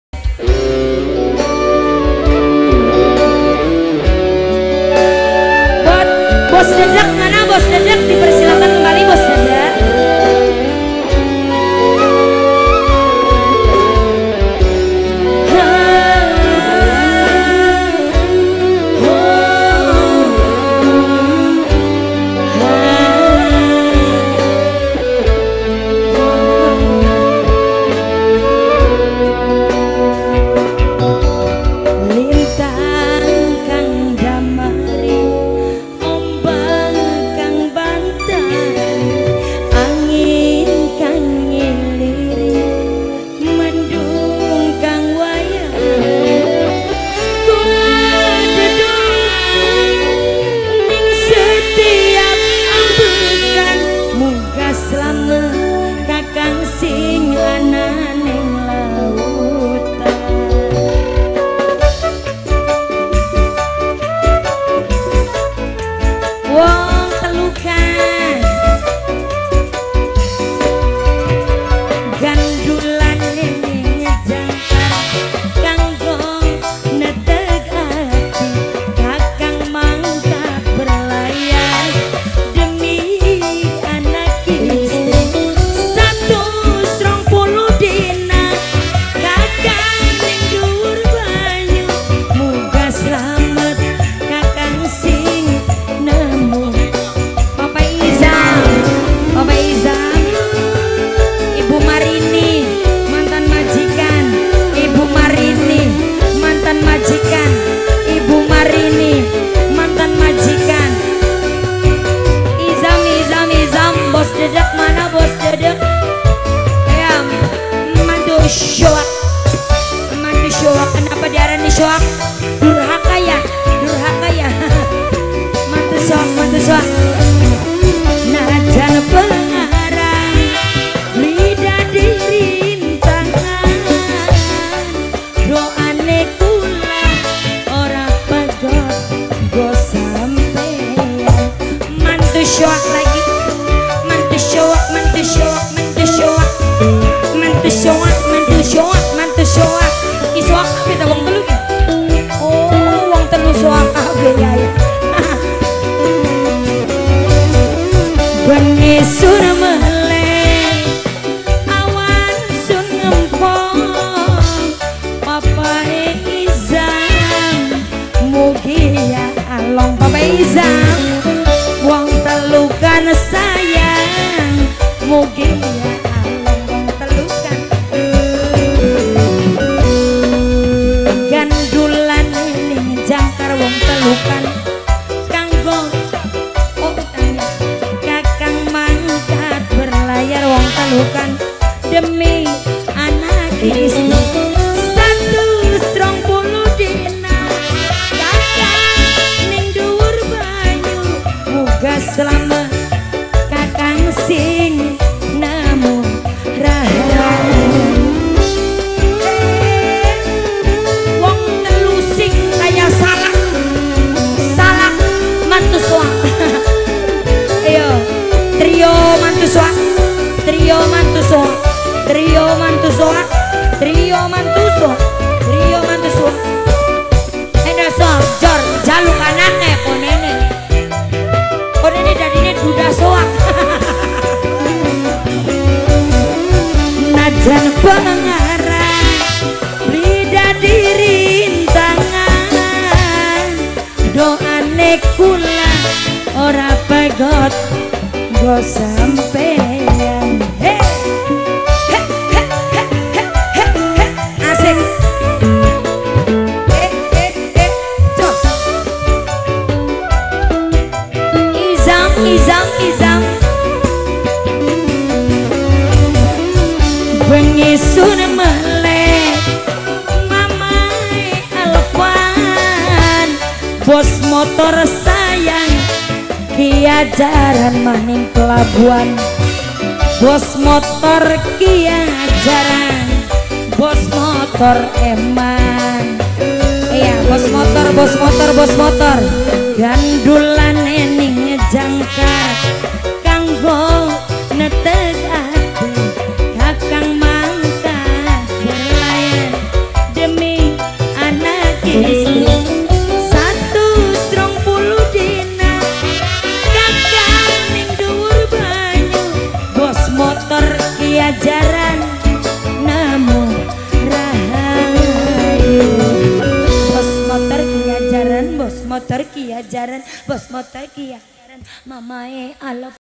SATUS 20 DINA SUSY ARZETTY LIVE KIAJARAN ACARA UNJUNGAN BUYUT 25 SEPTEMBER 2019 NIRWANA MANDALA
LIVE KIAJARAN ACARA UNJUNGAN BUYUT 25 SEPTEMBER 2019 NIRWANA MANDALA